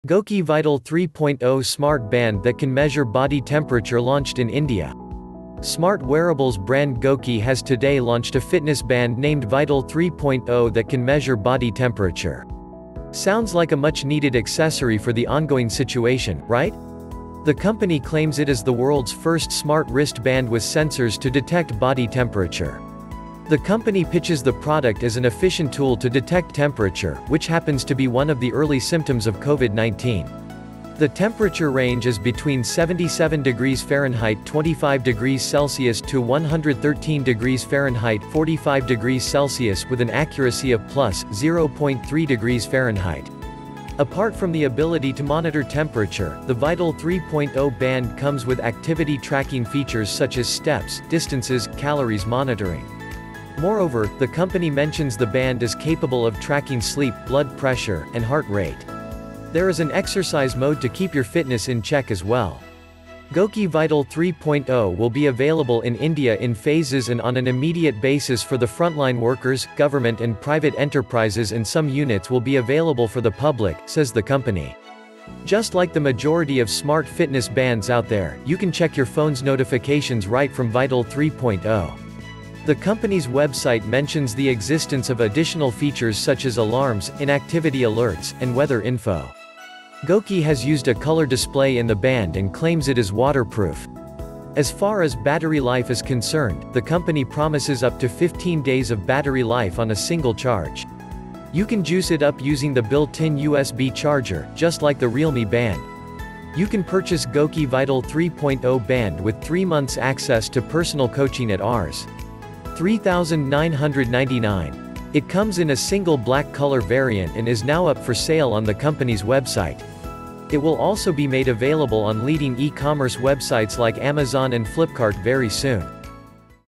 goqii vital 3 smart band measure body temperature launched india
Watch GOQii Vital 3.0 Smart Band That Can Measure Body Temperature Launched in India.